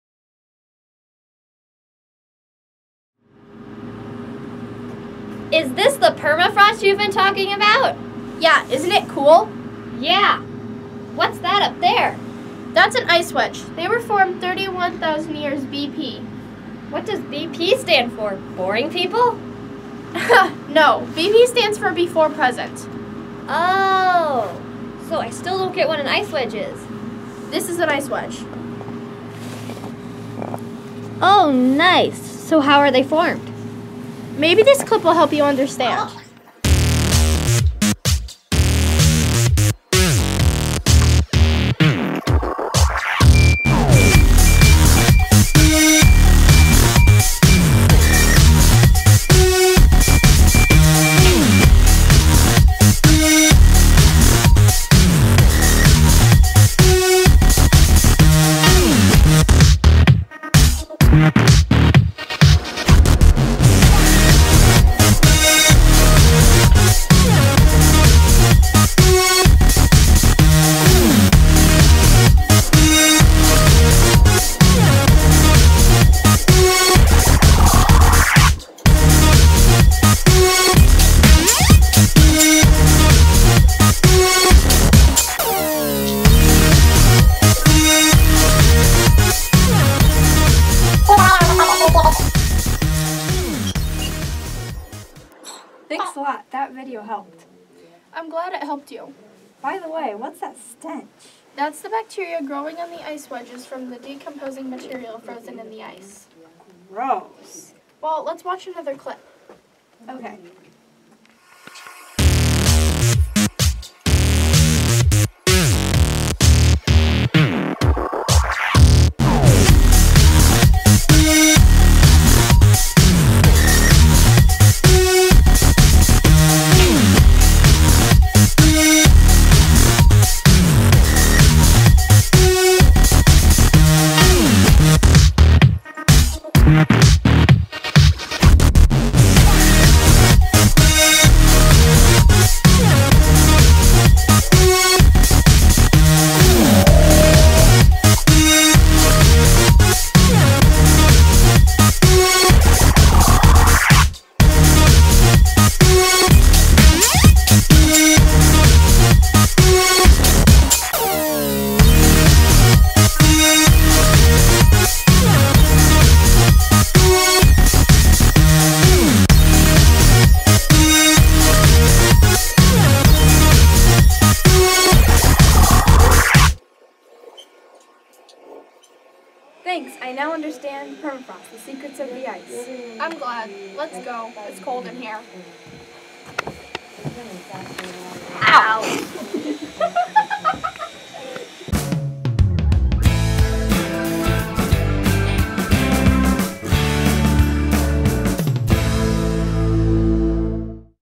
Ice Wedgies of Fungi
Skit about ice wedge formation and fungal and bacteria growth in the tunnel